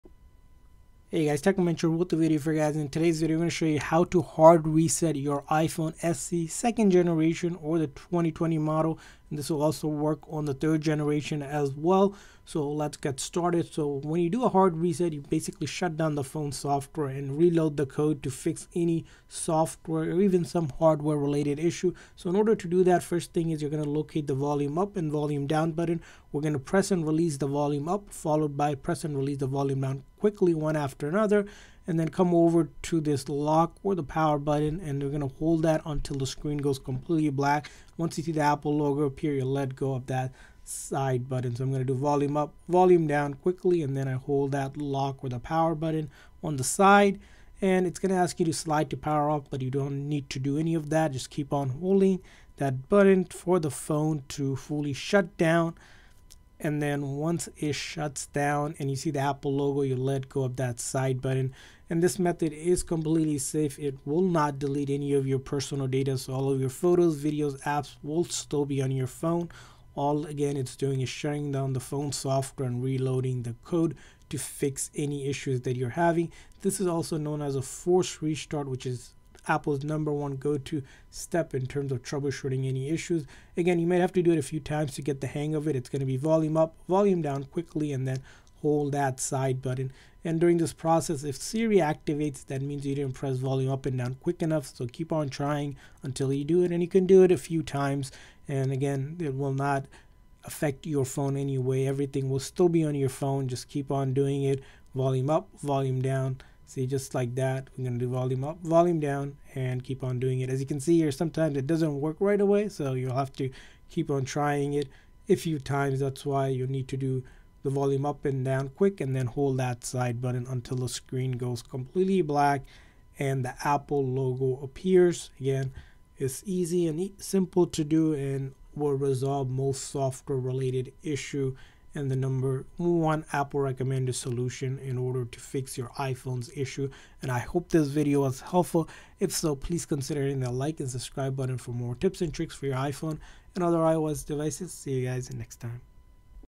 How to Hard Reset iPhone SE 2nd Generation
Is your iPhone SE 2nd Gen not working correctly? Then you will need to perform a hard reset! In this video we show you how to hard reset your Apple iPhone SE (2020) to resolve any issues. This method can be applied to newer iPhone SE 3rd Generation as well and will work even if the iPhone is frozen, stuck or unresponsive without losing any data.